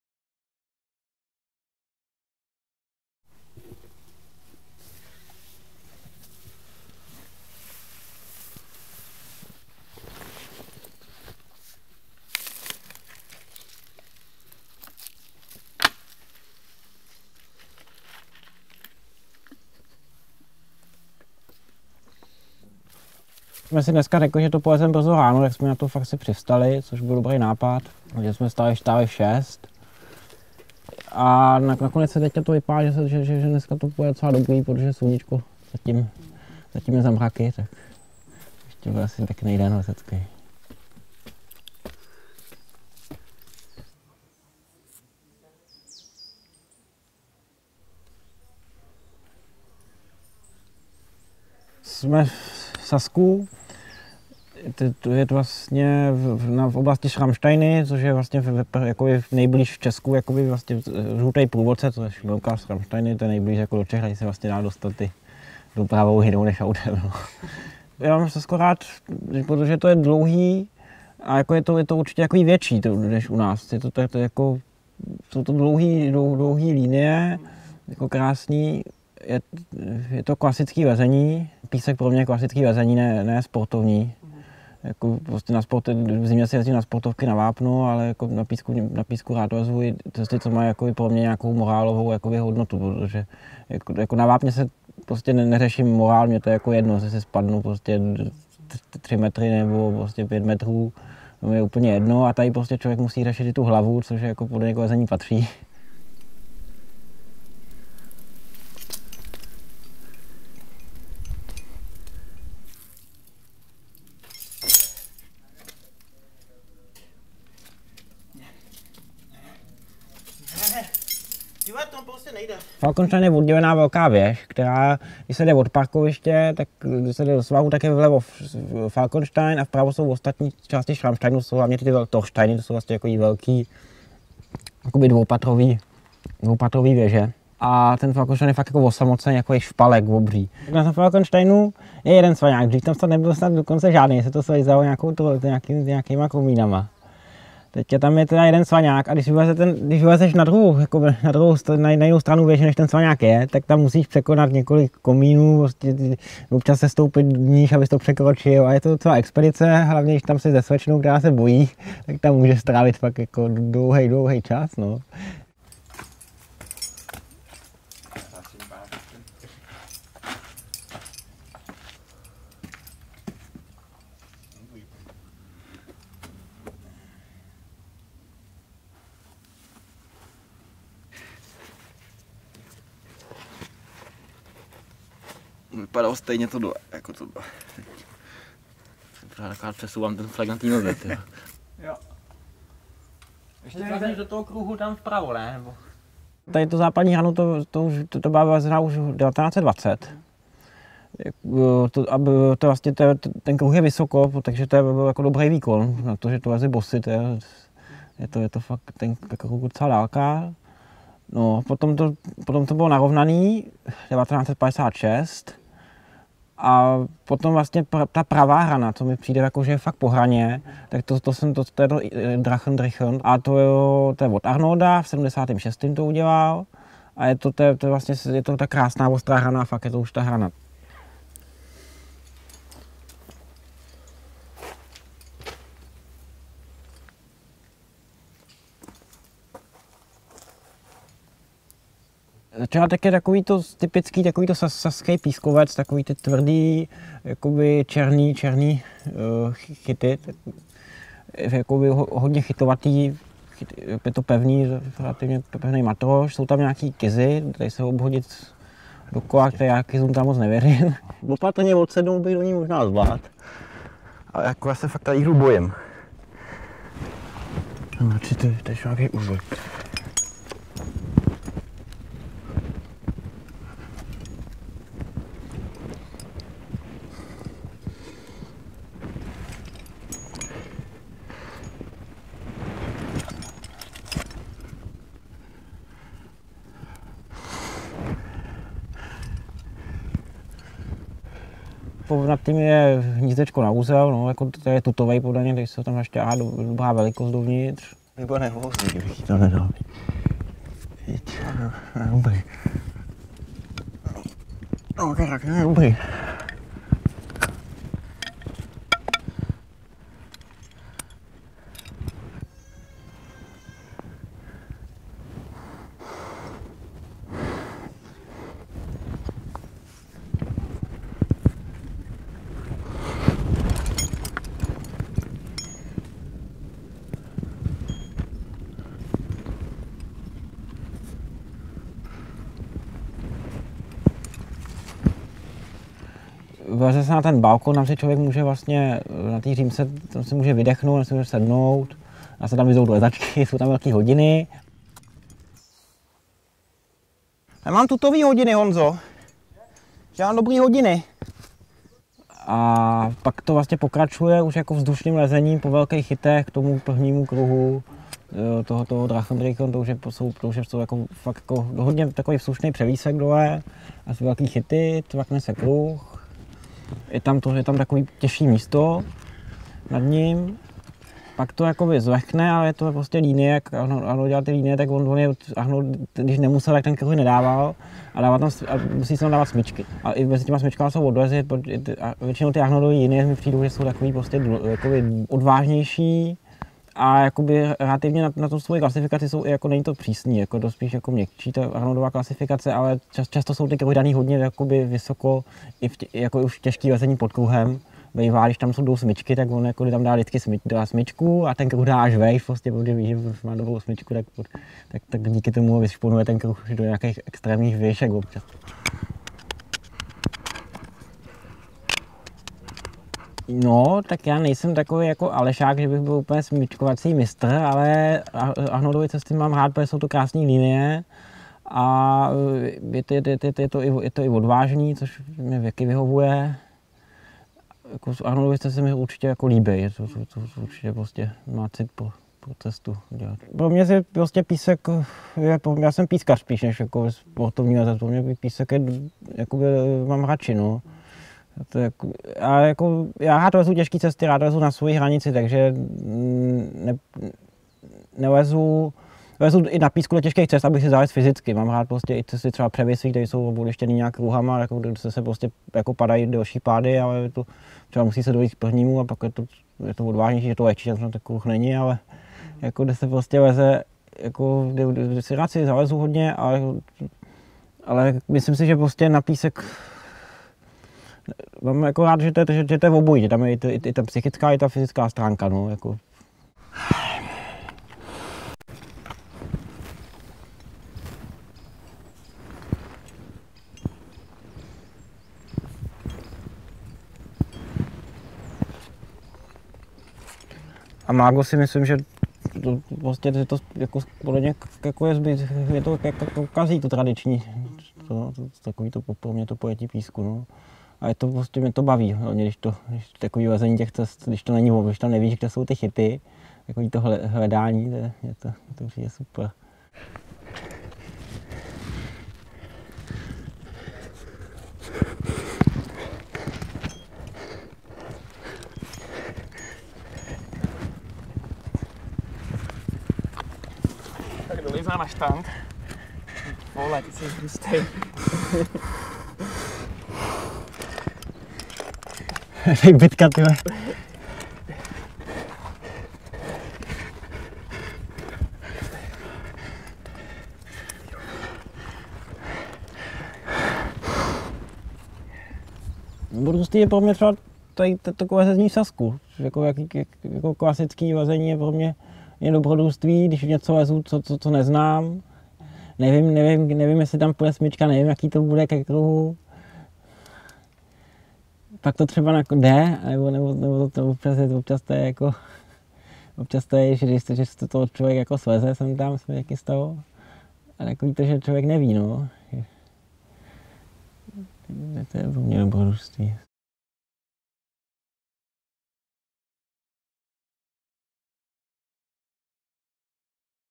Drachenrücken IXa, Sasko, leze Makak | eMontana.cz
+ENGLISH SUBTITLES
Leze Honza "Makak" Makovička. Přečti si i článek a mrkni na fotky